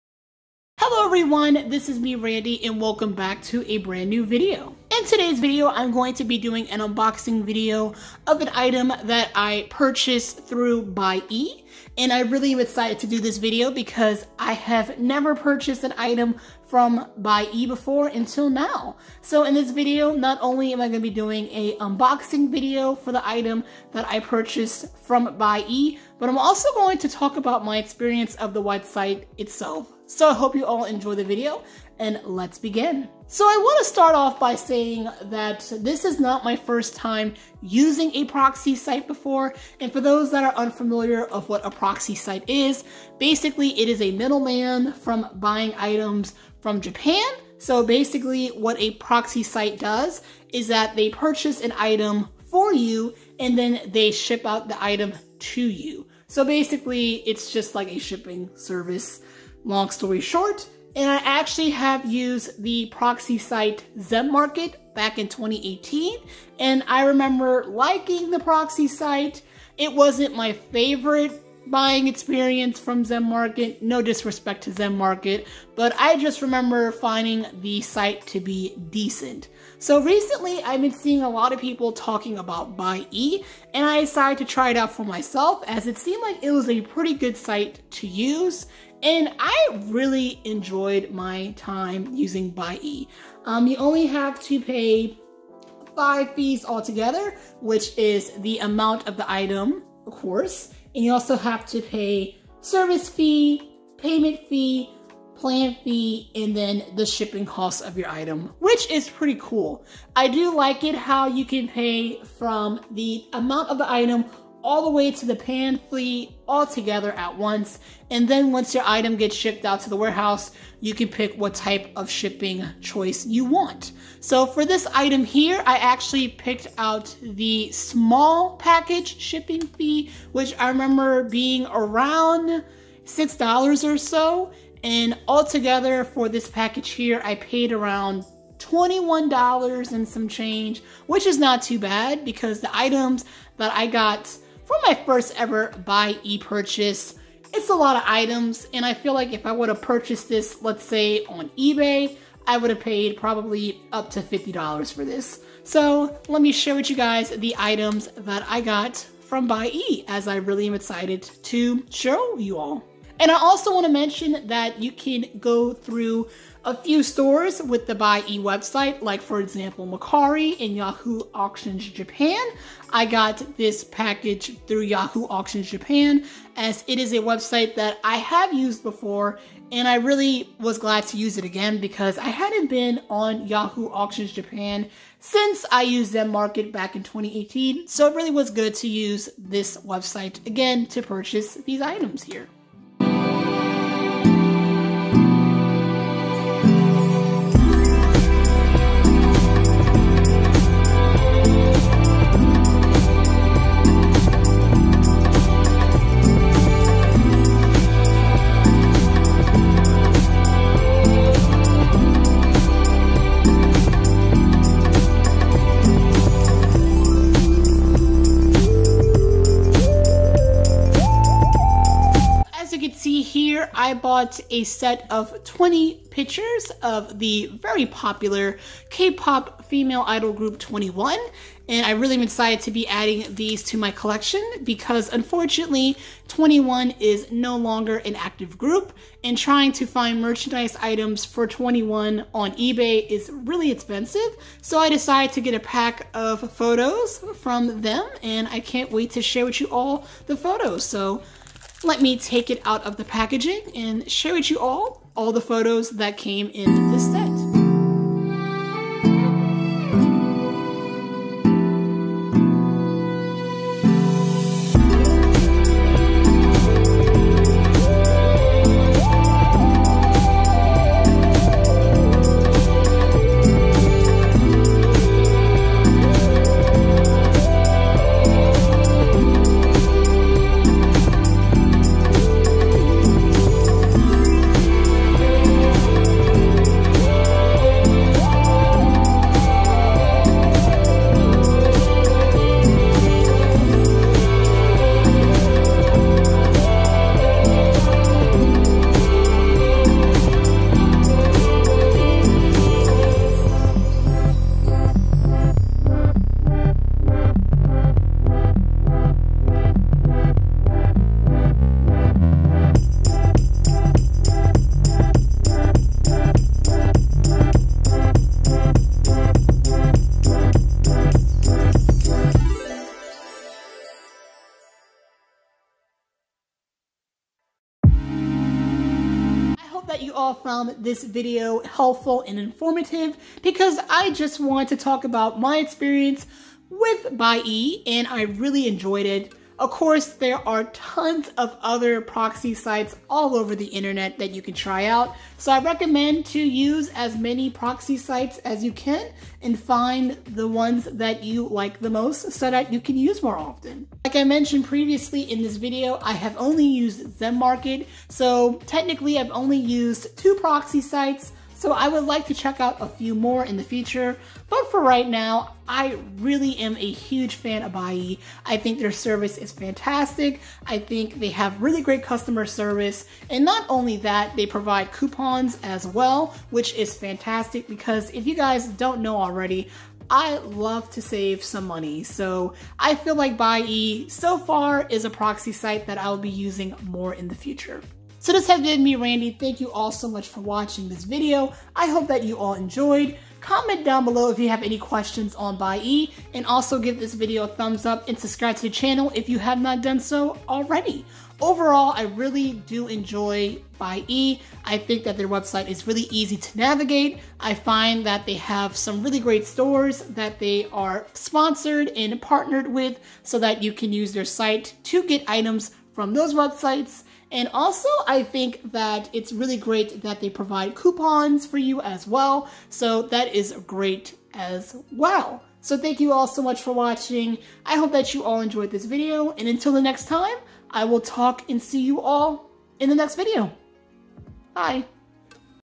|Perspective| My First Time Shopping on Buyee! {And Unboxing}
Here's a video that flimed back in February that I finally decided to uploaded!

I started to use Buyee recently and I decided to give out my thoughts and opinions on the site as well as share with you all my first ever purchase!

Since filming this video, I had purchased a few more items including a 2ne1 photocard from their second mini album!

I hope that you all find this video helpful!